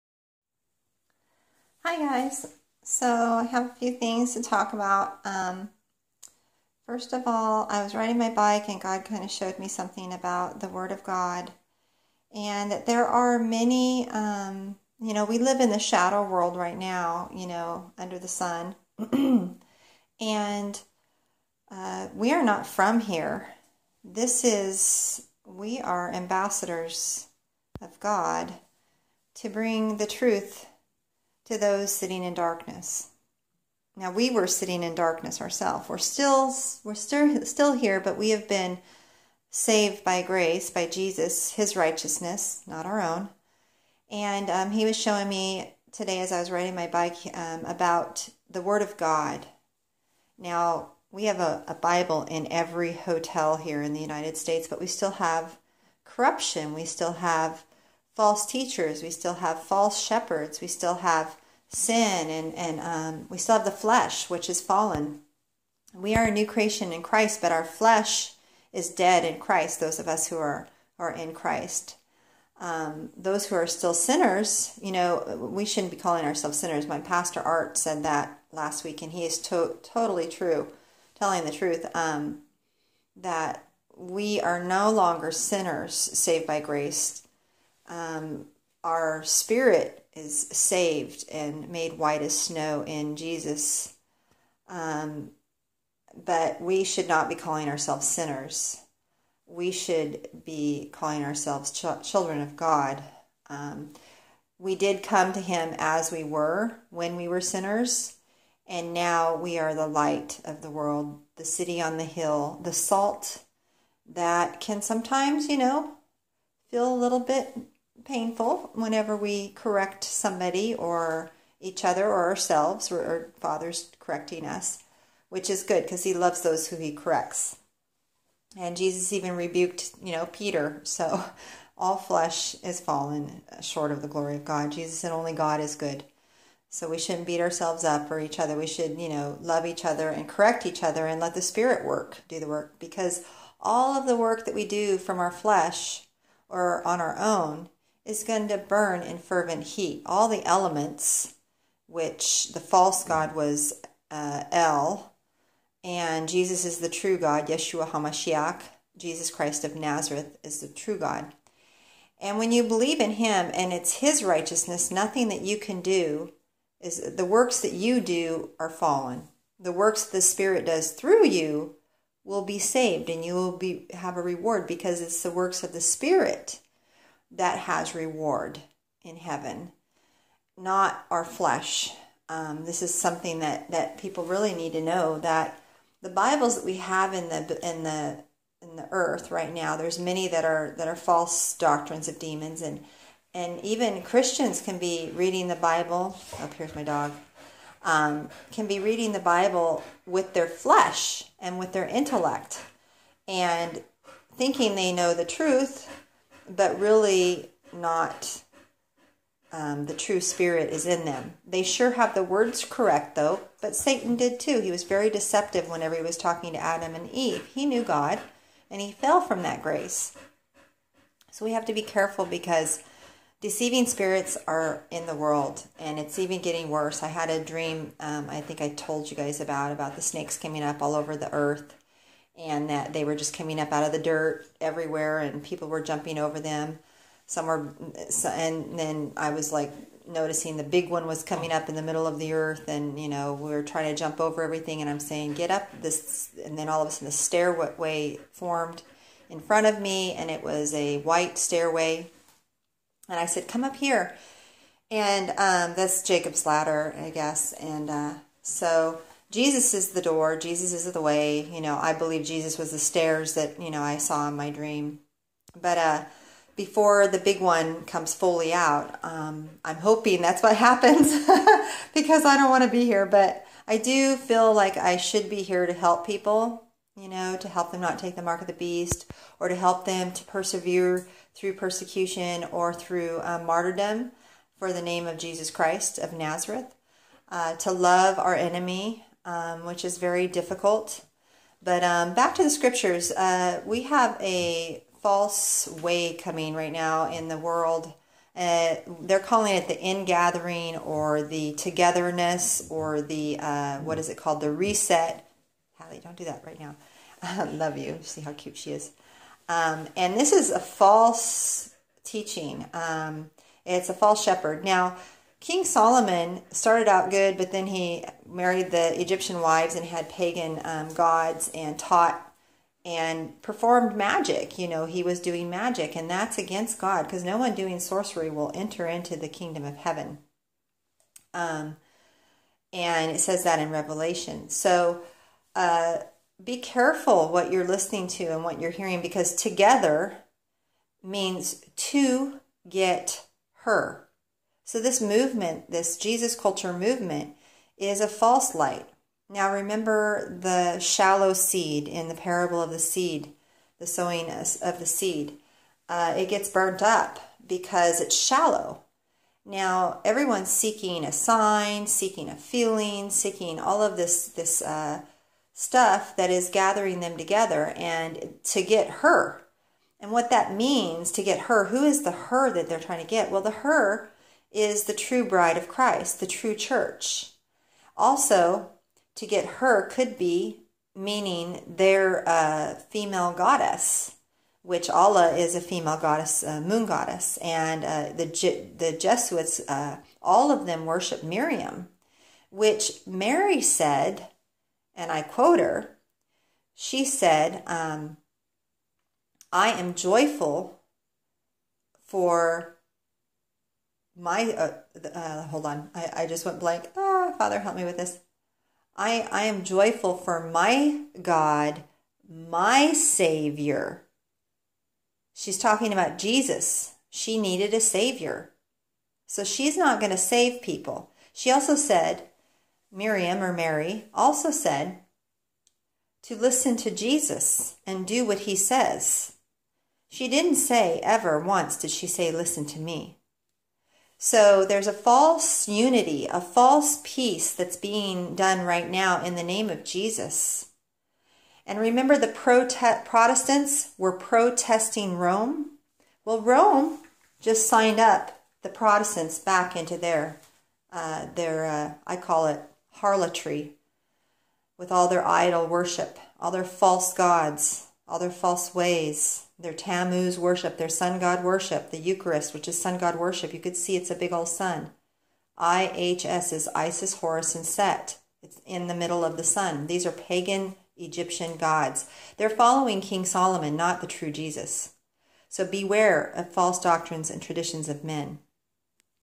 TRUMPET CALL OF GOD (JESUS)!
Beware of "together" (to-get-her" - The True Church) gathering of Rome/Vatican/Jesus Movement. Jesus said He came to DIVIDE! Many are following a false peace, brotherhood movement, in the name of jesus, love and mercy, which is a false light. The light that leads to darkness!

Jesus said He will send the angels to gather us, not people. He also said to not go when people say, "jesus is here or Jesus is there."

Many wolves in sheep's clothing leading the flock over a cliff. They seek a man or gathering that is not found in His Word.

There's a gathering in Washington (pagan city) that has a serpent symbol that is a false light. These people seek a sign, a feeling or a show. That isn't the same Jesus of His Word.

Ask Jesus Christ of Nazareth to show you the Truth about all of this cry for a "reset" or "brotherly love." Seek Him!